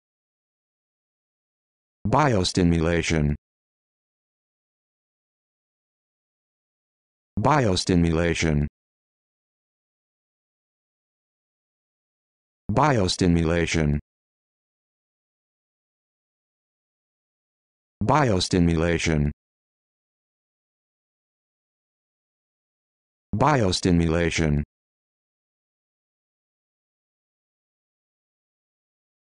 How to Pronounce Biostimulation
This video shows you how to pronounce Biostimulation